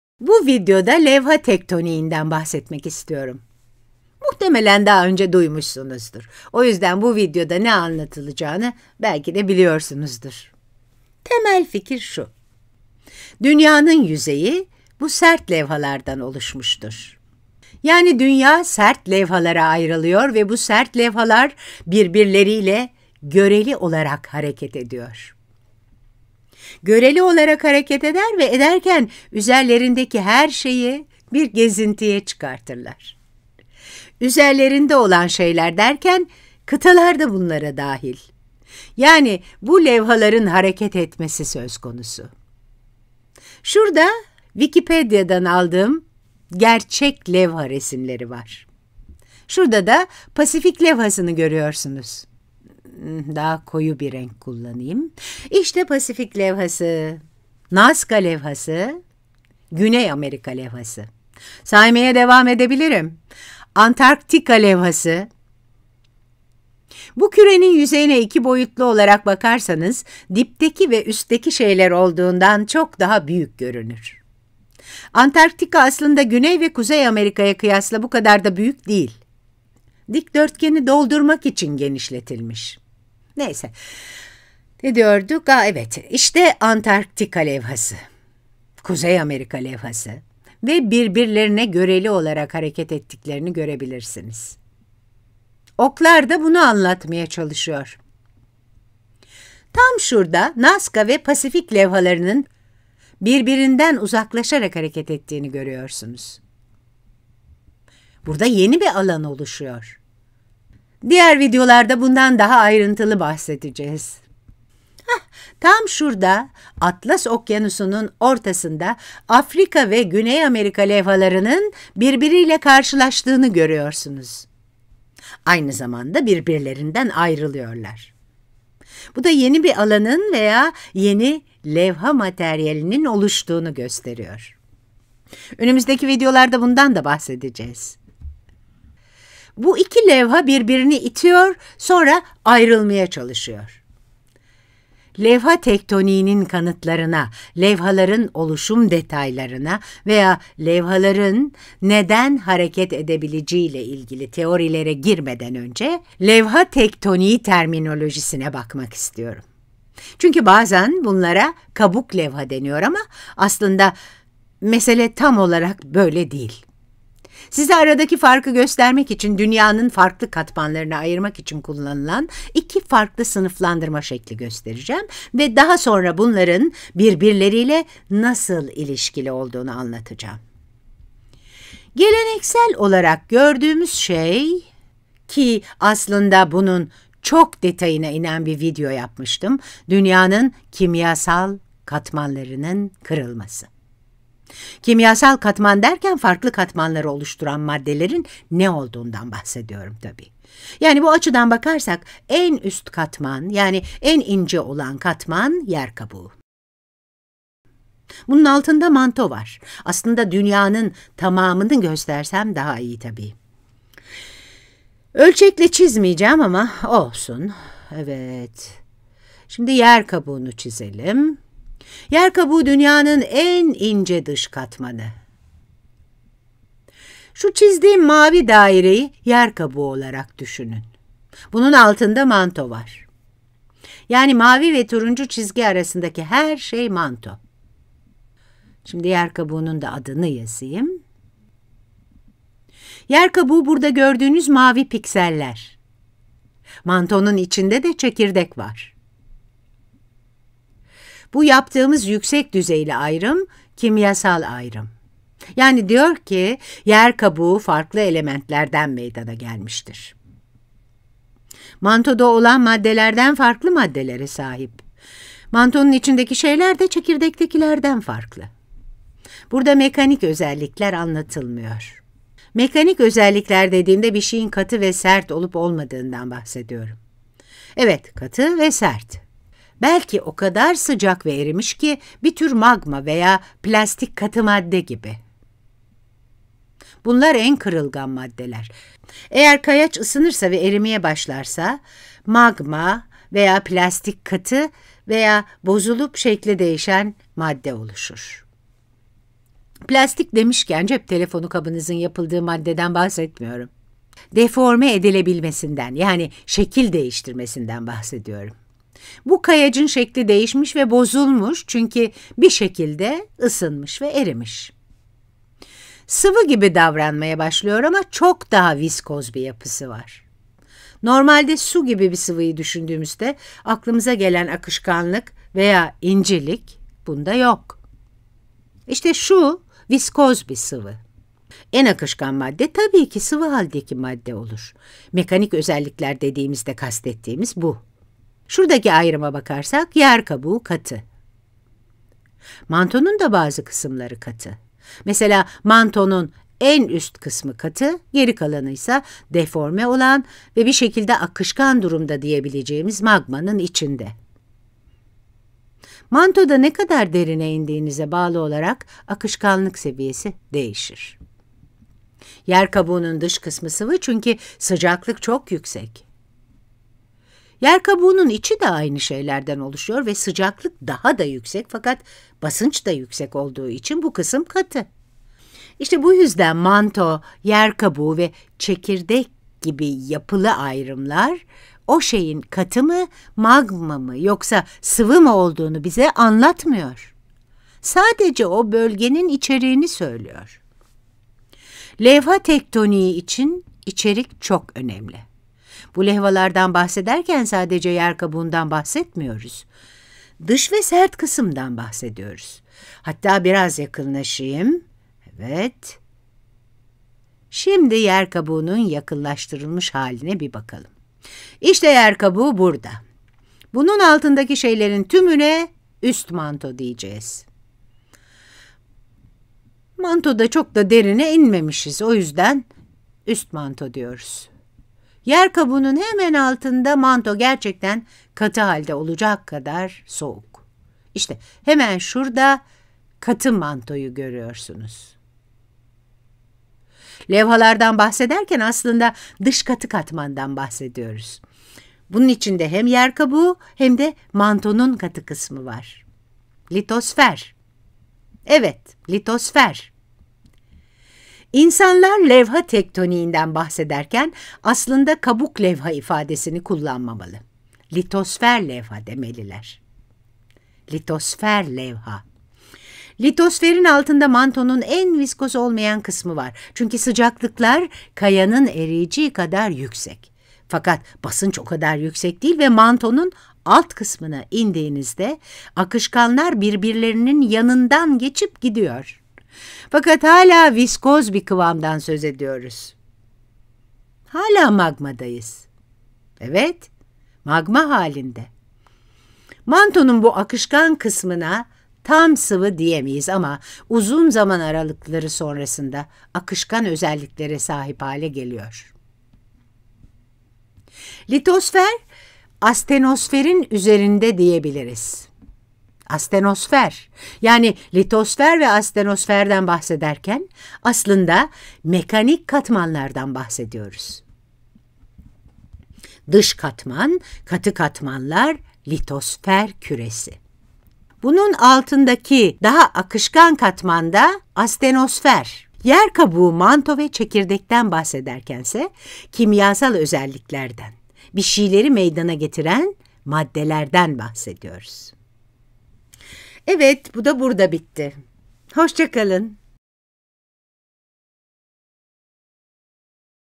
Levha Tektoniği: Yer Kabuğu ve Litosferin Farkı (Yer ve Uzay Bilimleri)
Plate Tectonics: Difference between crust and lithosphere